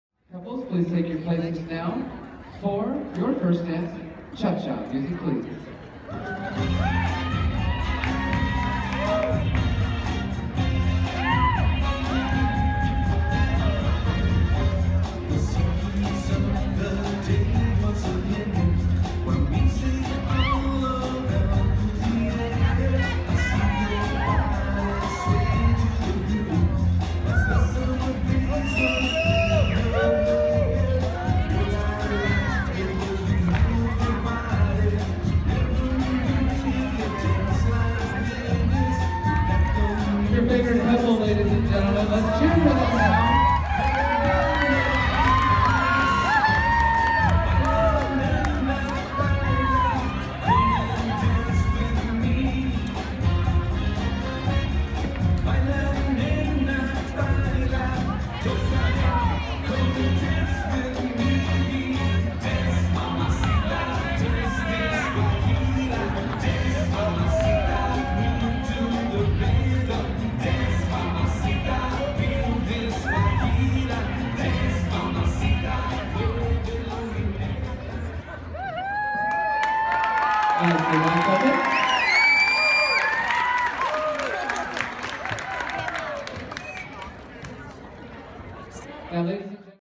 Junior Open Rhythm Cha Cha Final at Provo High - Jan 29
Caleb & Ashley at finals for Junior Open II Rhythm (Cha Cha/Swing/Bolero/Mambo) at Provo High Dancesport competition. They got 2nd place.